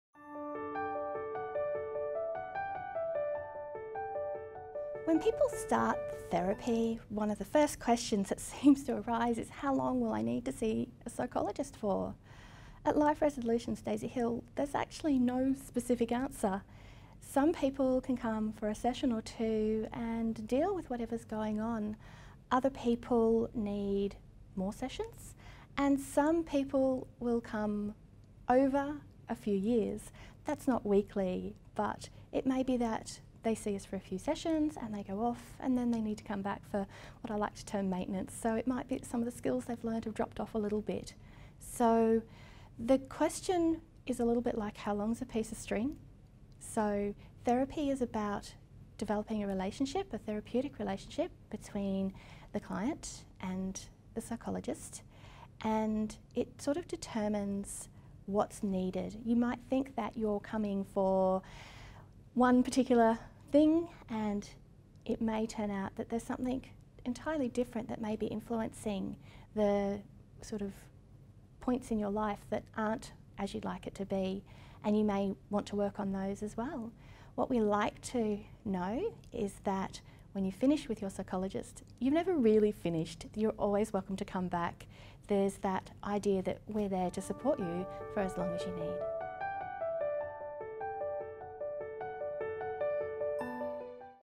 Belinda Bayliss, Life Resolutions Daisy Hill Psychologist: "What is therapy about?"
To find out more about our services and how we can help, visit: